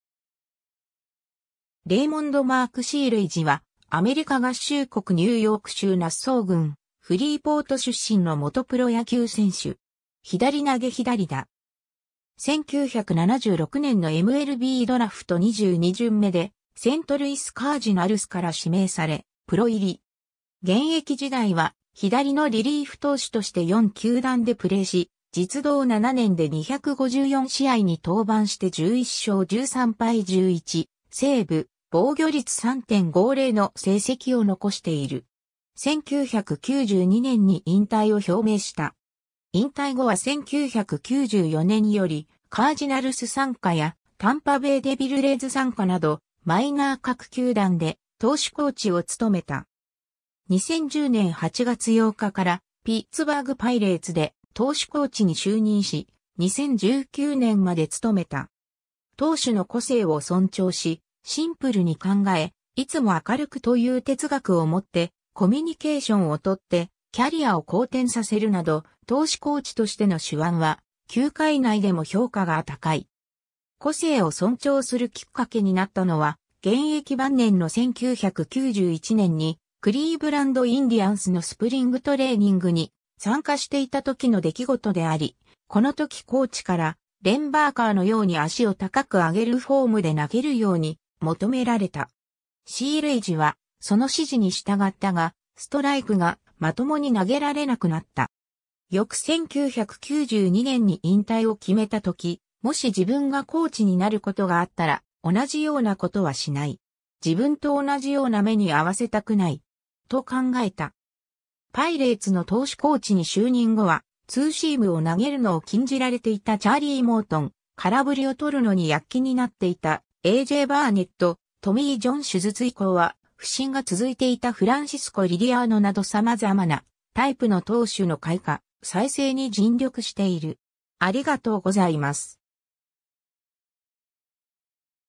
レイ・シーレイジ
セントルイス・カージナルス傘下の選手
ニューヨーク・メッツ傘下の選手
クリーブランド・インディアンス傘下の選手
ミルウォーキー・ブルワーズ傘下の選手
ロサンゼルス・ドジャース傘下の選手
レイモンド・マーク・シーレイジ（Raymond Mark Searage, 1955年5月1日 - ）は、アメリカ合衆国ニューヨーク州ナッソー郡フリーポート出身の元プロ野球選手（投手）。
左投左打。
 1976年のMLBドラフト22巡目（全体518位）でセントルイス・カージナルスから指名され、プロ入り。
現役時代は左のリリーフ投手として4球団でプレーし、実働7年で254試合に登板して11勝13敗11セーブ・防御率3.50の成績を残している。
1992年に引退を表明した。
 引退後は1994年よりカージナルス傘下やタンパベイ・デビルレイズ傘下などマイナー各球団で投手コーチを務めた。
2010年8月8日からピッツバーグ・パイレーツで投手コーチに就任し、2019年まで務めた。
 投手の個性を尊重し、「シンプルに考え、いつも明るく」という哲学を持ってコミュニケーションを取ってキャリアを好転させるなど、投手コーチとしての手腕は球界内でも評価が高い。
 個性を尊重するきっかけになったのは、現役晩年の1991年にクリーブランド・インディアンスのスプリングトレーニングに参加していた時の出来事であり、この時コーチからレン・バーカーのように脚を高く上げるフォームで投げるように求められた。
シーレイジはその指示に従ったが、ストライクがまともに投げられなくなった。
翌1992年に引退を決めた時、「もし自分がコーチになることがあったら、同じようなことはしない。自分と同じような目に遭わせたくない。」と考えた。
 パイレーツの投手コーチに就任後は、ツーシームを投げるのを禁じられていたチャーリー・モートン、空振りを取るのに躍起になっていたA.J.バーネット、トミー・ジョン手術以降は不振が続いていたフランシスコ・リリアーノなど様々なタイプの投手の開花・再生に尽力している。